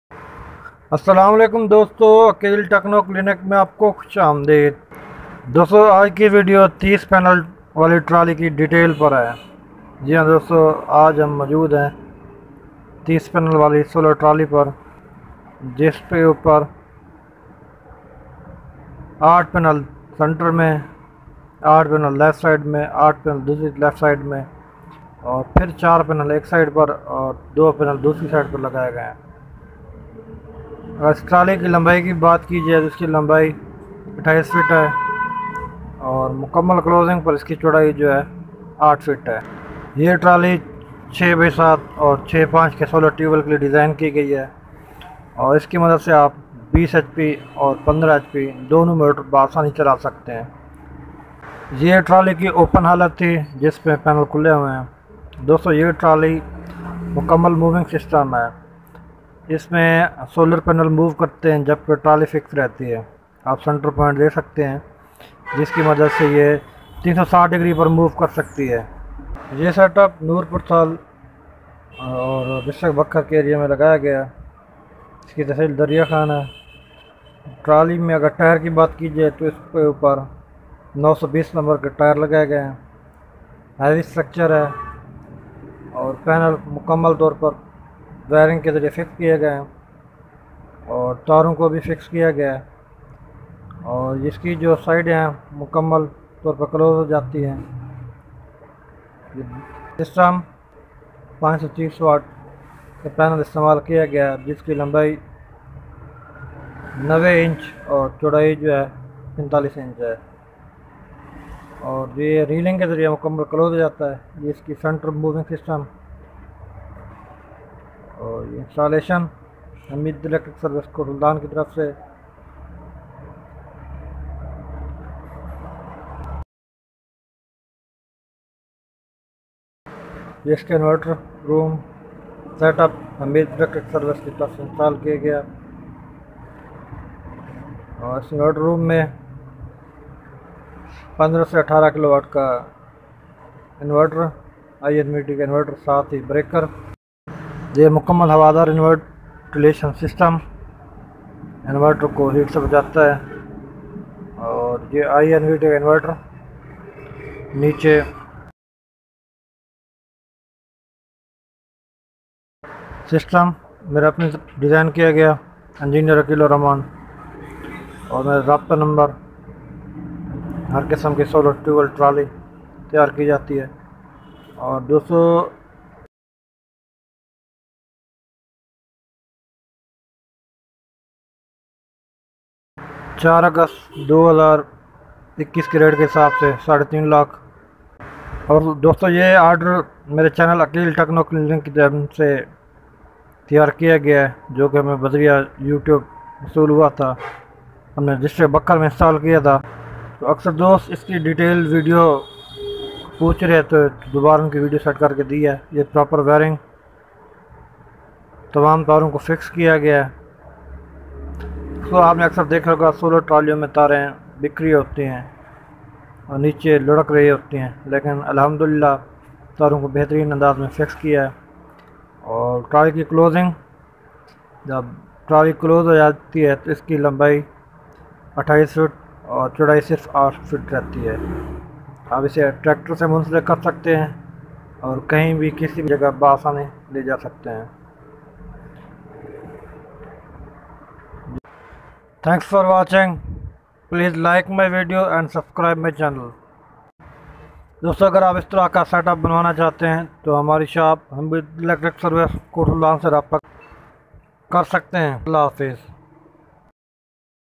Moveable Solar folding trolley 30 panels|Solar System Trolley 30 Panels|SolarTrali Stand 30 Panel
This video is about Solar Folding Trolley Frame With 30 panel of Jinko A Grade Solar Panels of 530 watt. Solar Trolley Stand is totally  moveable for all kind of solar system.Its price on August 4,2021 is only350000/-rupees.